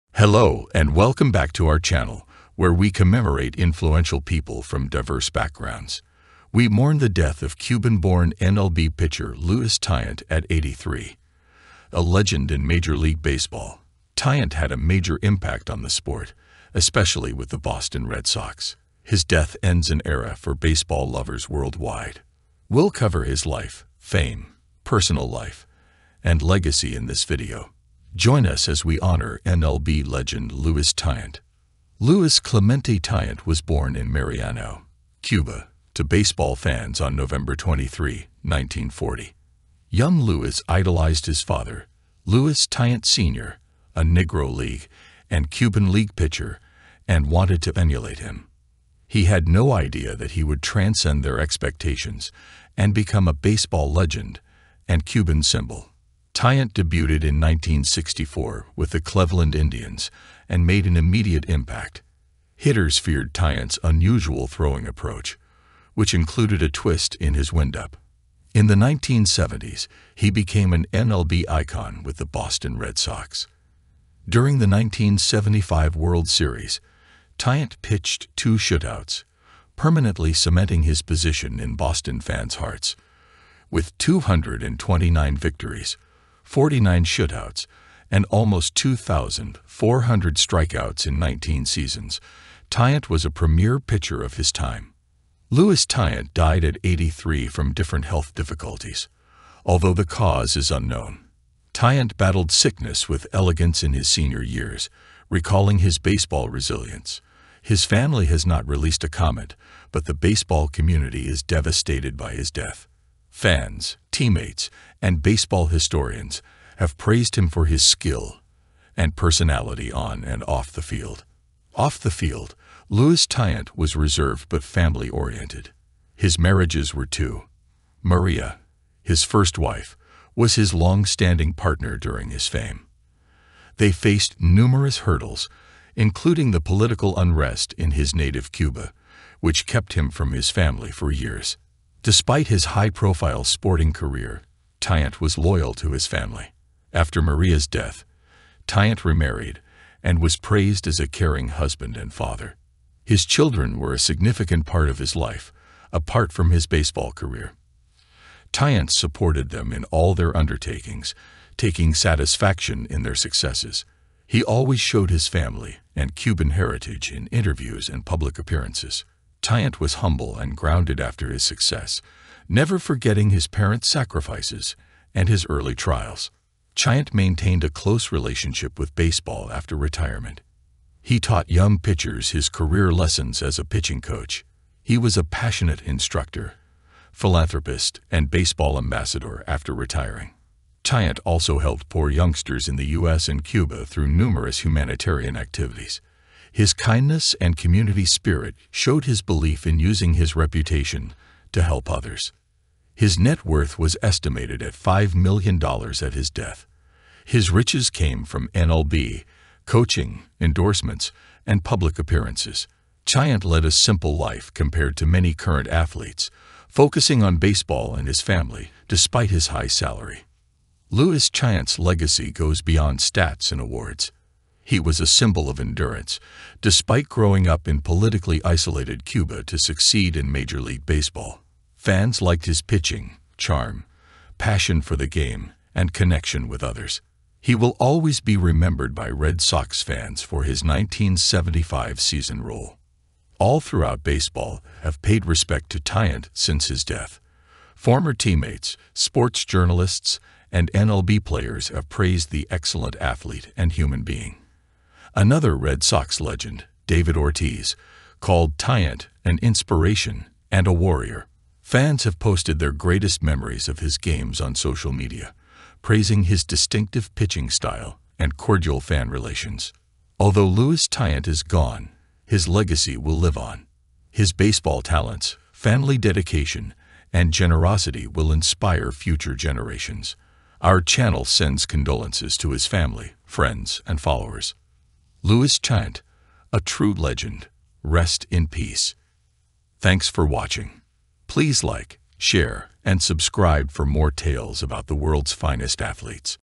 Farewell and obituary to a Legend Luis Tiant’s : Luis Tiant’s Remarkable Journey in MLB
Today, we mourn the passing of legendary MLB pitcher Luis Tiant, who died at 83. Born in Cuba, Tiant followed in his father’s footsteps to become a baseball icon, most notably with the Boston Red Sox. Known for his unique pitching style and his role in the 1975 World Series, Tiant’s legacy in baseball is undeniable. With 229 victories and nearly 2,400 strikeouts in his 19-season career, he left a lasting mark on the sport.

Off the field, Tiant was a humble, family-focused man who valued his Cuban heritage. Despite the political challenges that kept him away from his homeland for years, Tiant remained devoted to his family and his roots. After retirement, he continued to contribute to baseball by mentoring young pitchers and engaging in charitable work, supporting underprivileged youth in the US and Cuba.

Luis Tiant’s legacy will live on through his incredible career, his devotion to family, and his generosity. Our thoughts go out to his family, friends, and fans. Luis Tiant, rest in peace.